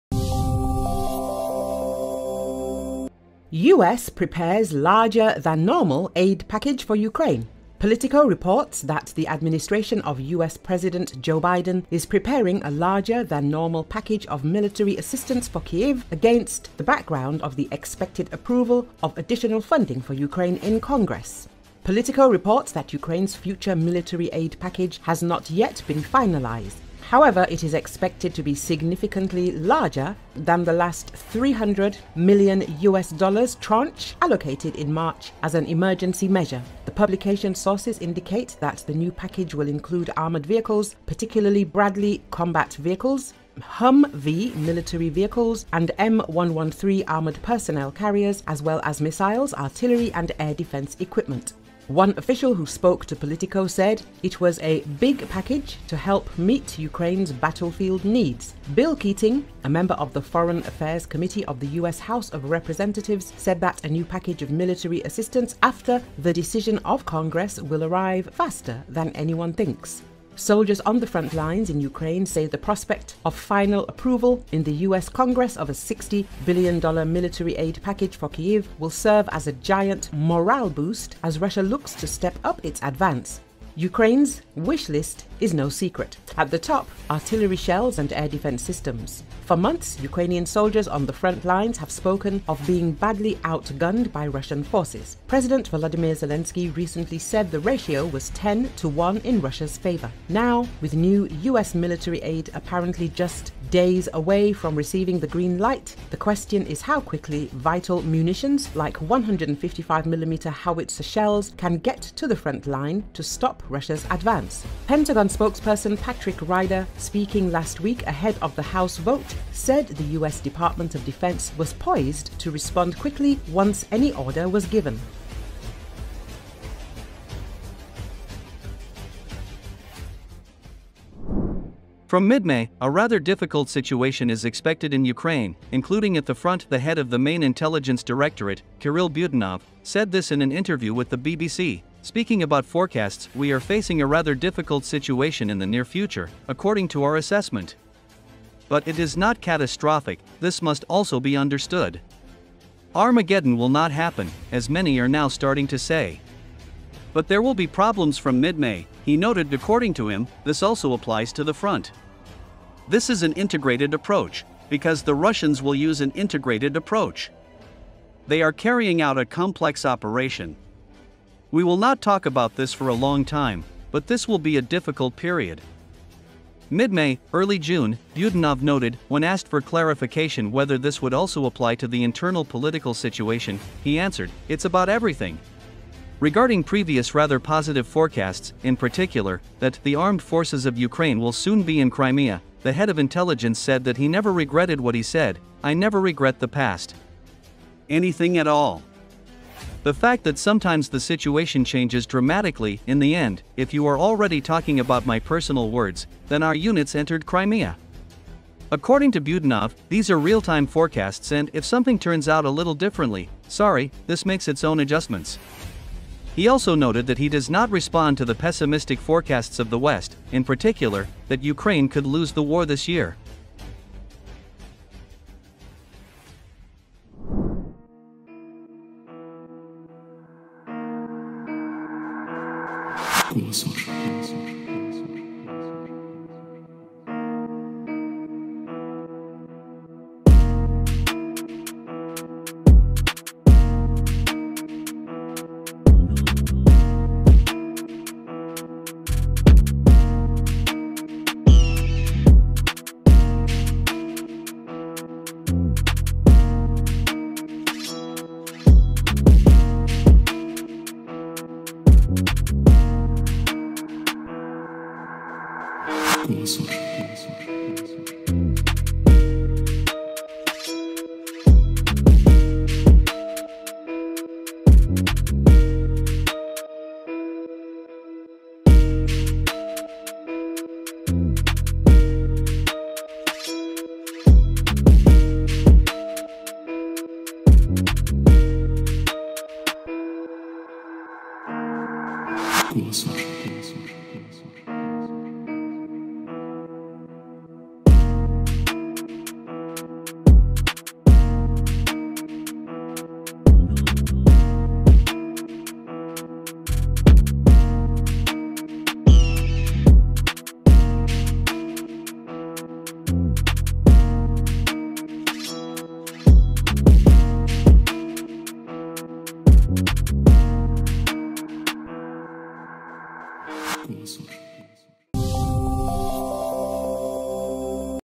US prepares "larger than normal" aid package for Ukraine
Politico reports that the administration of US President Joe Biden is preparing a "larger than normal" package of military assistance for Kyiv against the background of the expected approval of additional funding for Ukraine in Congress.
 Politico reports that Ukraine's future military aid package has not yet been finalised. However, it is expected to be "significantly larger" than the last US$300 million tranche allocated in March as an emergency measure.
The publication's sources indicate that the new package will include armoured vehicles, particularly Bradley combat vehicles, Humvee military vehicles and M113 armoured personnel carriers, as well as missiles, artillery and air defence equipment.
One official who spoke to Politico said it was a "big package to help meet Ukraine’s battlefield needs".
Bill Keating, a member of the Foreign Affairs Committee of the US House of Representatives, said that a new package of military assistance after the decision of Congress will arrive "faster than anyone thinks".
Soldiers on the front lines in Ukraine say the prospect of final approval in the US Congress of a $60 billion military aid package for Kyiv will serve as a giant morale boost as Russia looks to step up its advance. Ukraine’s wish list is no secret. At the top: artillery shells and air defense systems. For months, Ukrainian soldiers on the front lines have spoken of being badly outgunned by Russian forces. President Volodymyr Zelensky recently said the ratio was 10 to one in Russia’s favor. Now, with new US military aid apparently just days away from receiving the green light, the question is how quickly vital munitions like 155mm Howitzer shells can get to the front line to stop Russia’s advance. Pentagon spokesperson Patrick Ryder, speaking last week ahead of the House vote, said the US Department of Defense was “poised to respond quickly,” once any order was given.